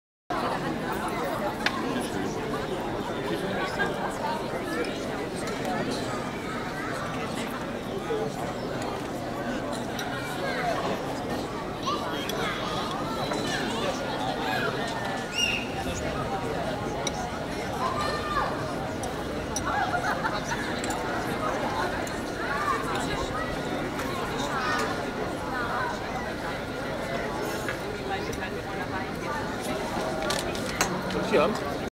Heidelberg Market place #1
Sunday in the Marktplaz in Heidelberg Germany. Sit, have something to drink and eat and watch the tourists watching other tourists. The Heidelberg Castel sits high above overlooking this center of Germany's romantic city. The Holy Ghost Church towers over all.